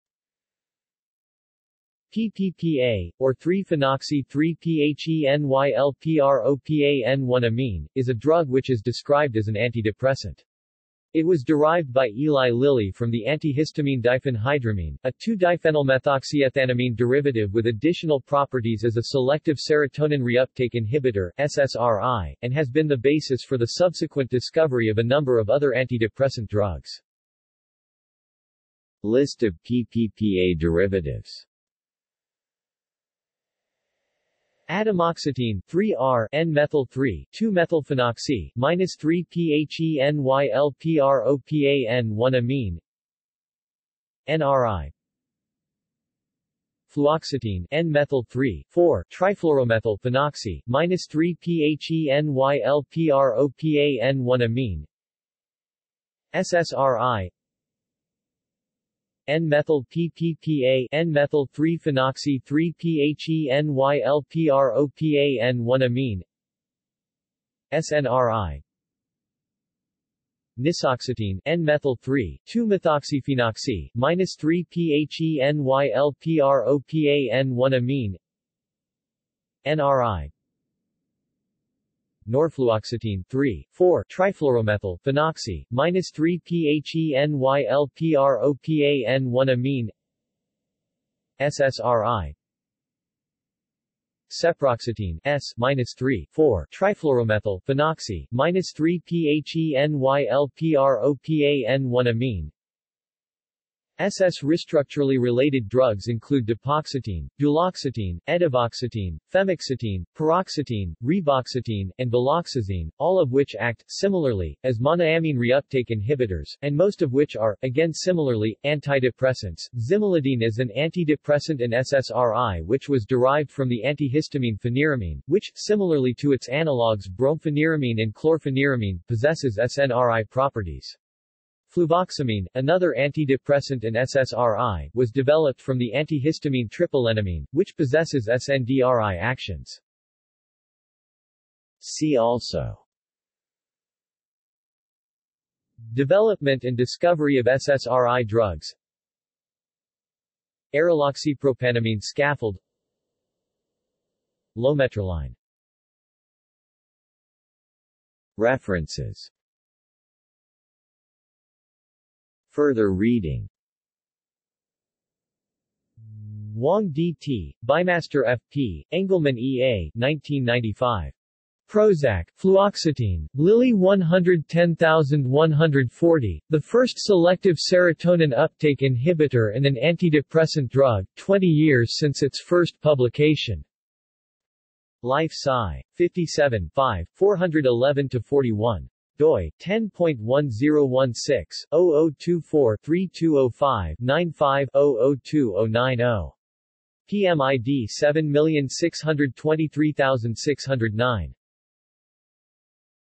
PPPA (drug) | Wikipedia audio article
This is an audio version of the Wikipedia Article:
PPPA (drug)

SUMMARY
PPPA, or 3-phenoxy-3-phenylpropan-1-amine, is a drug which is described as an antidepressant. It was derived by Eli Lilly from the antihistamine diphenhydramine, a 2-diphenylmethoxyethanamine derivative with additional properties as a selective serotonin reuptake inhibitor (SSRI), and has been the basis for the subsequent discovery of a number of other antidepressant drugs.